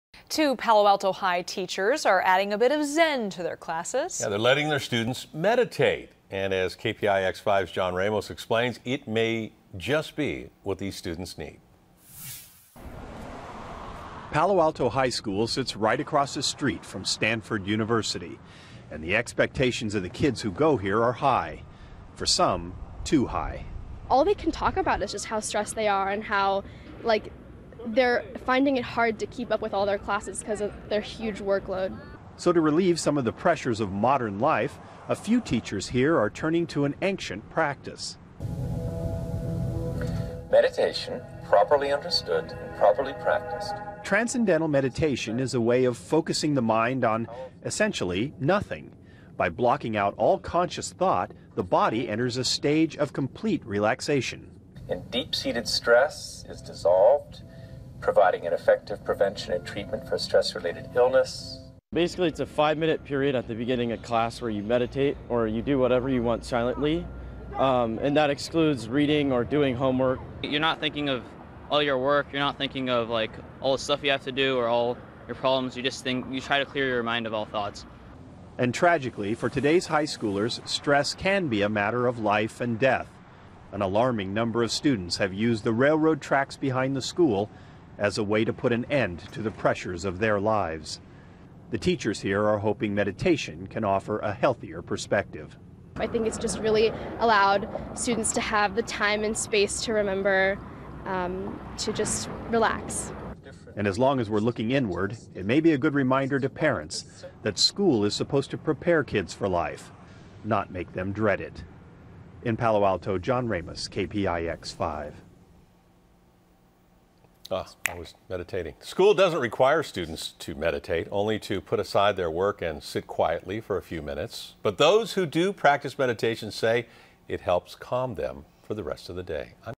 Meditation Helping Stressed Out Students At Palo Alto High School
Meditation is helping stressed out students at a Silicon Valley high school unwind during class. John Ramos reports.